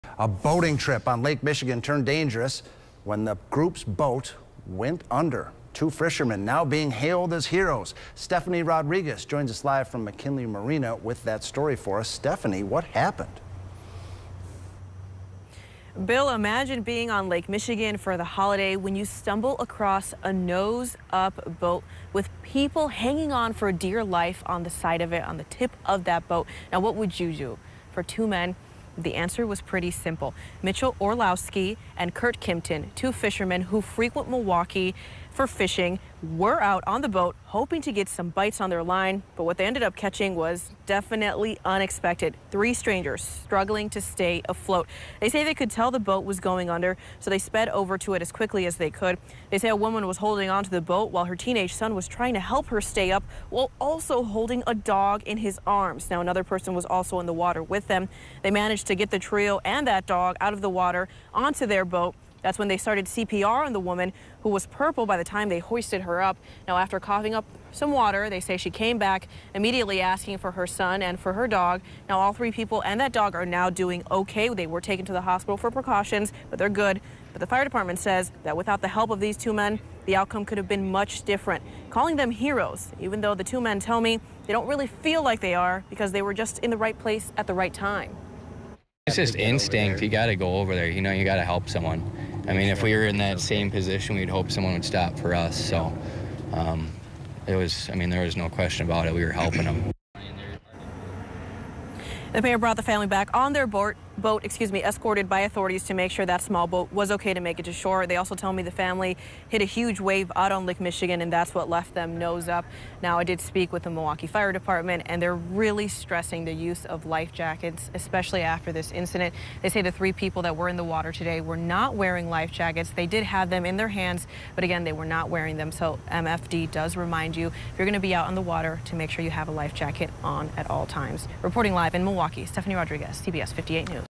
'It's just instinct': Fishermen save family on Lake Michigan
Two fishermen are being hailed as heroes after rescuing three people on Lake Michigan.